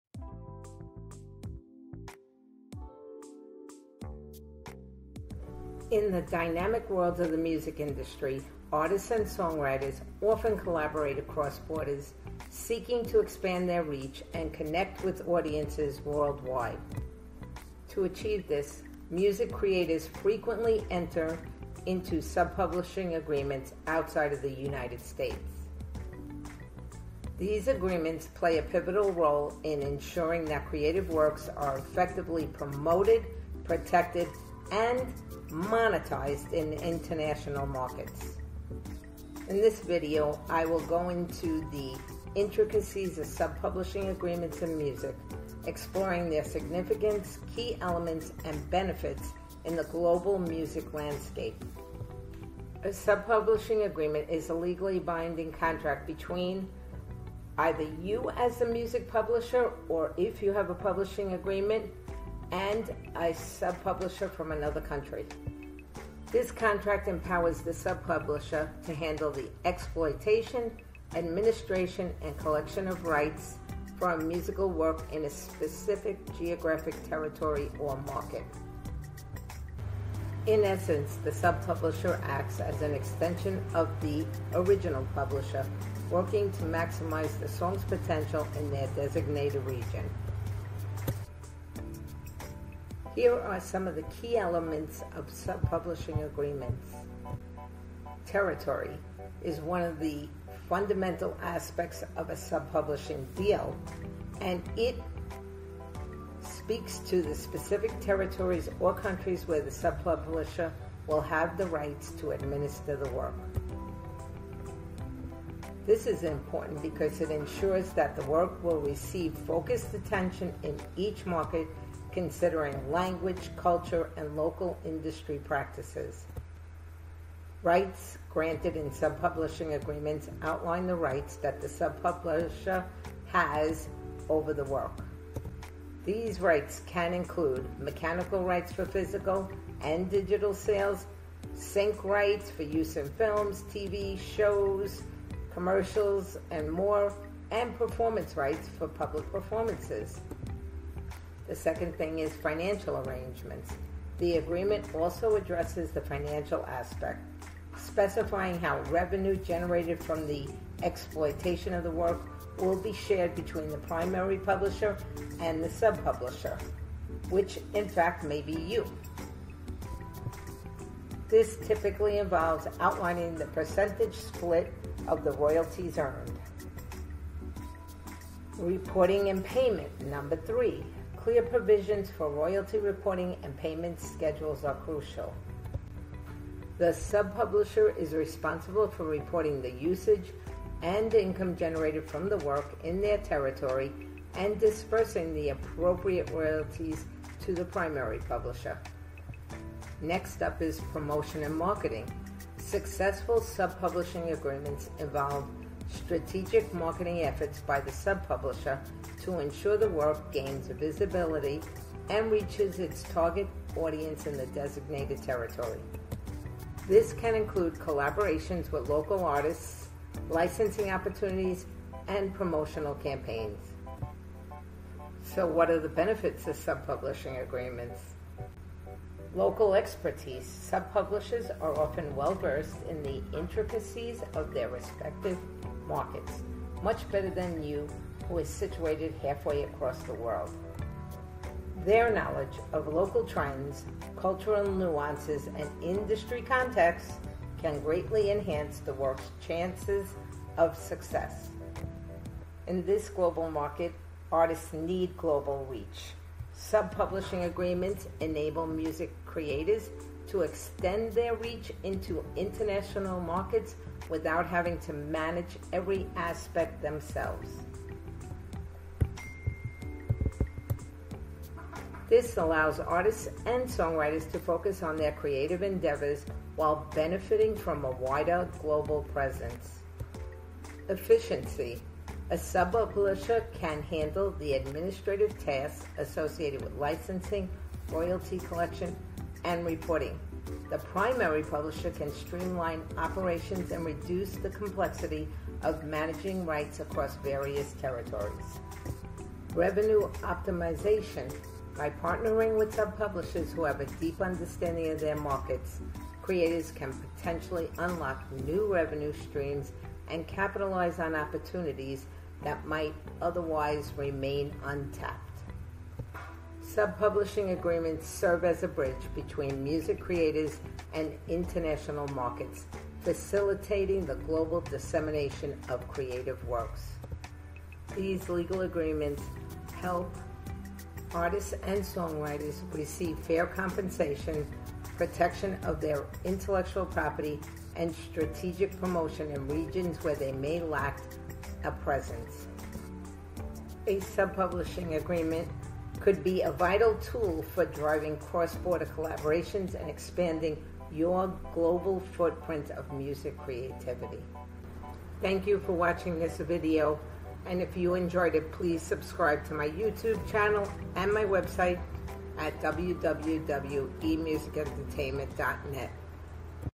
Navigating Sub-publishing Agreements in International Music Markets
Does growing a global audience and making money interest you?  Sub publishing can help you do both of those things.

If you are an Indie Artist, Label or Manager there are some very important and specific reasons why you need to understand the music business.  Most importantly you are spending time, energy and money so you need to know how to generate revenue.  Learn about sub-publishing and how it can help you earn money and grow a global audience.

And if you want to learn more go to my